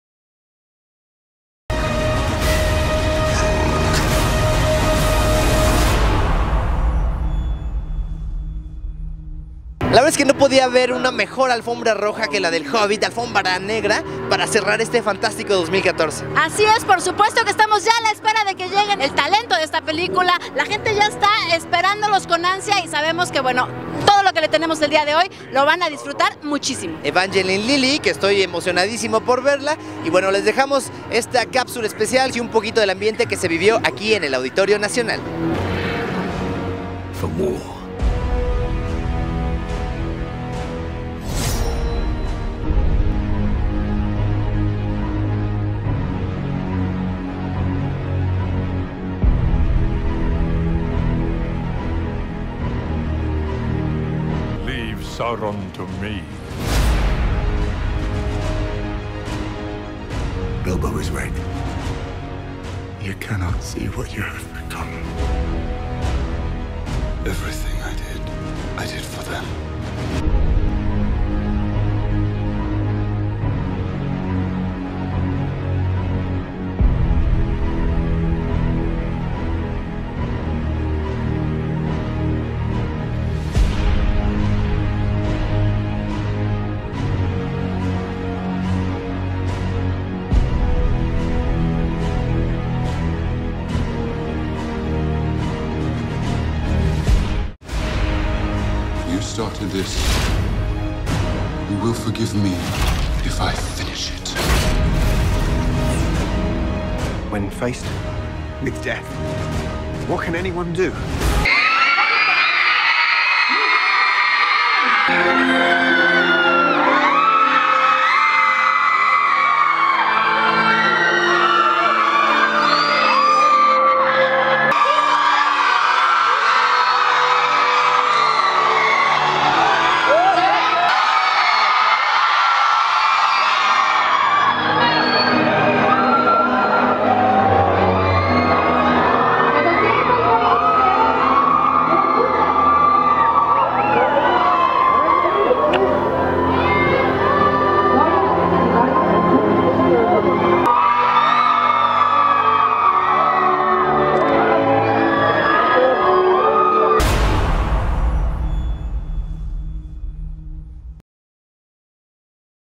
El Hobbit / Especial de la Premiere
Les dejamos una cápsula con los personajes que se dieron cita en el Auditorio Nacional para celebrar el fin de la película
La Batalla de los Cinco Ejércitos.
Conducen Salvador Franco y Daniela Nuño